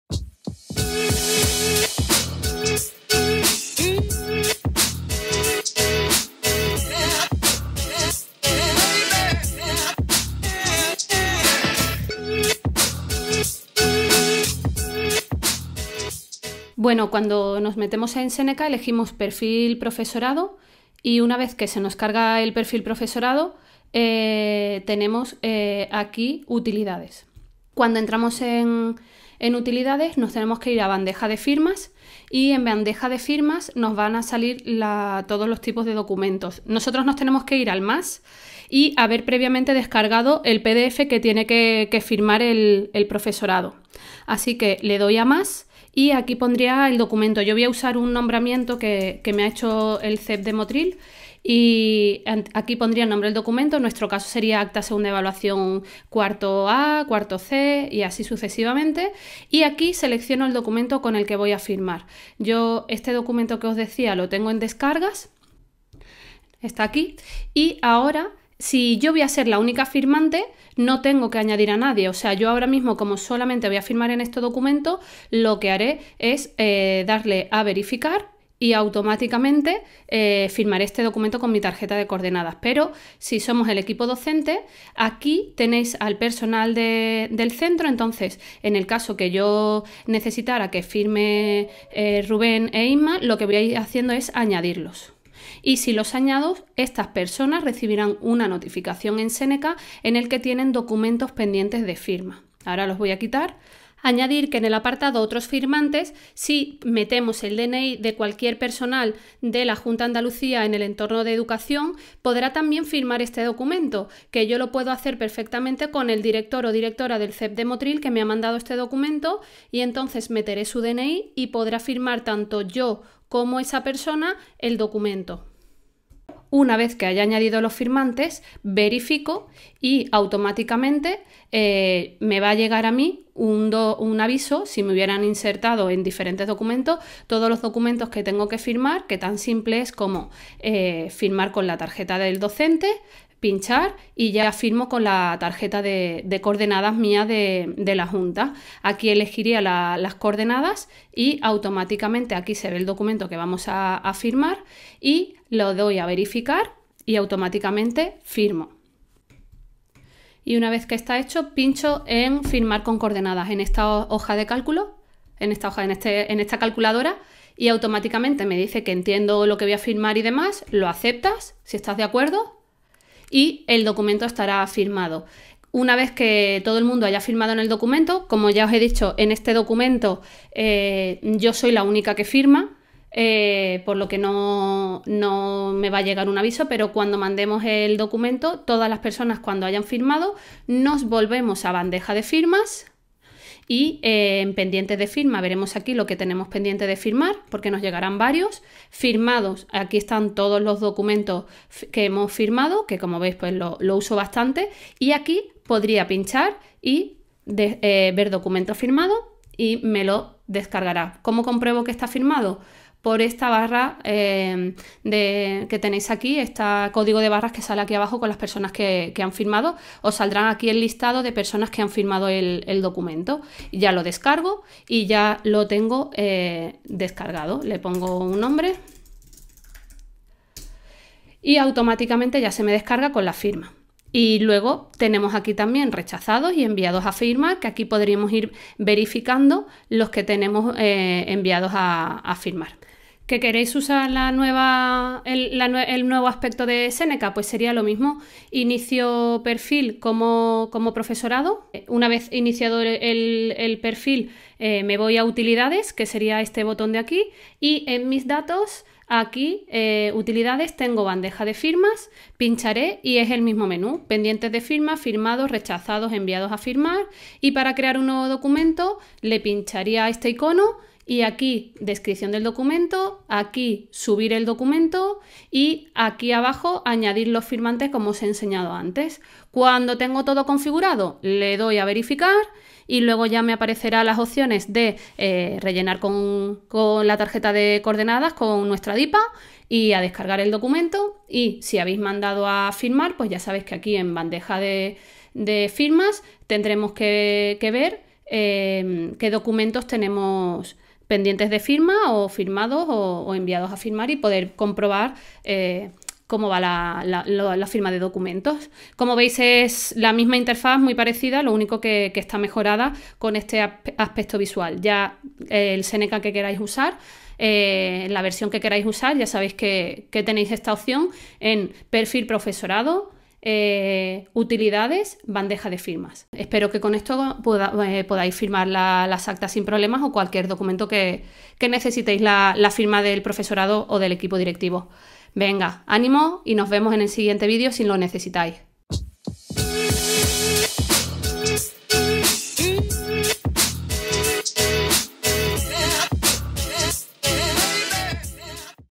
Cómo firmar documentos en Séneca varias personas
El profesorado de la Junta de Andalucía puede firmar documentos en Séneca de forma digital y telmática. En este vídeotutorial explico cómo se hace tanto en la versión antigua de Séneca como en la nueva. Además, puedes ver que no solamente puedes firmar documentos con el profesorado de tu centro, sino con el personal de la Junta de Andalucía si tienes su DNI.